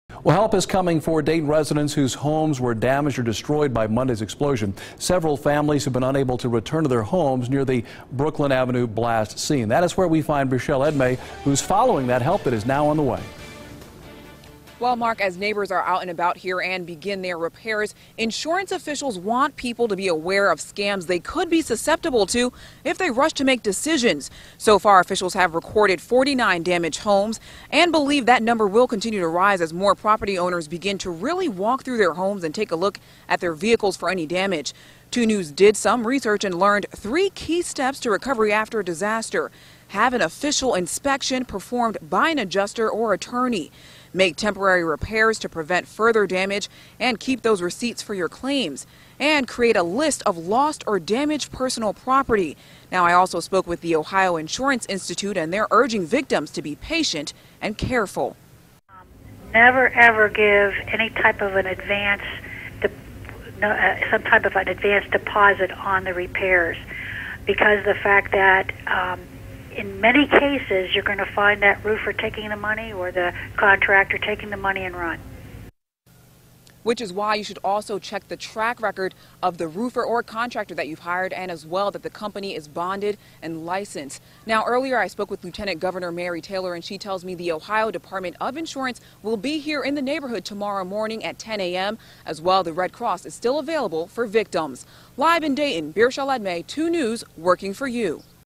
Dayton explosion site under investigation
Some families still out of their homes